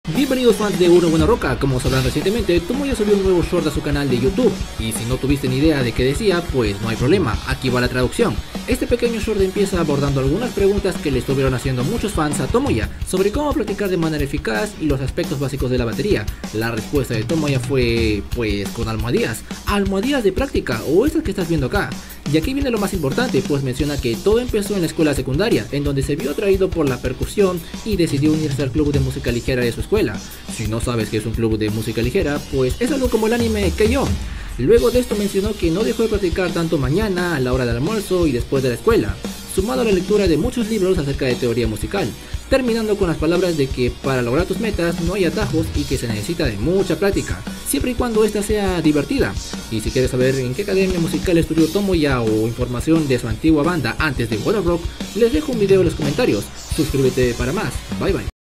Tomoya Kanki nos habla de su vida escolar y sus inicios en la batería!
Mañana con suerte se hablará y traducirá el especial del Primal Footmark, donde nos hablan de las nuevas canciones del album.

Sacado de :
▬▬▬▬▬▬▬▬▬▬▬▬▬▬▬▬▬▬۩۞۩ஜ▬▬▬▬▬▬▬▬▬▬▬▬▬▬▬▬▬▬

Si te gusto el contenido o algún concierto que traduje, apóyame en streamlabs para ponerle pan a mi desayuno. xD
Gracias!!
▬▬▬▬▬▬▬▬▬▬▬▬▬▬▬▬▬▬۩۞۩ஜ▬▬▬▬▬▬▬▬▬▬▬▬▬▬▬▬▬▬